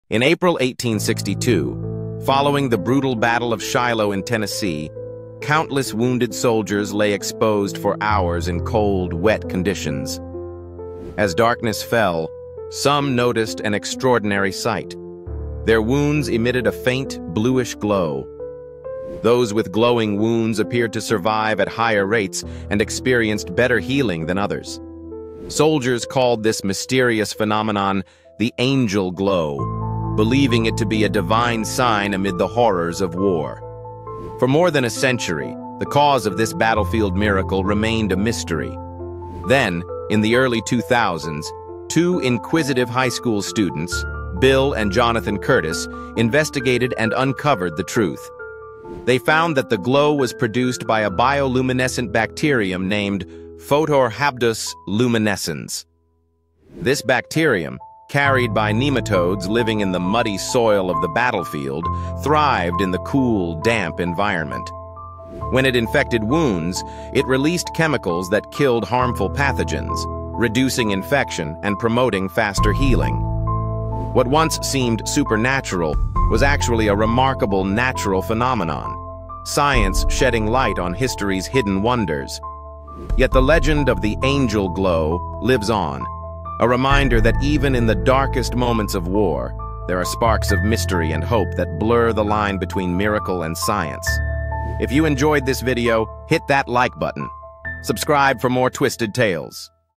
Angel Glow : The Mysterious Battlefield Miracle of Shiloh
Discover the eerie 'Angel Glow' that saved soldiers at Shiloh. Science explains this Civil War mystery!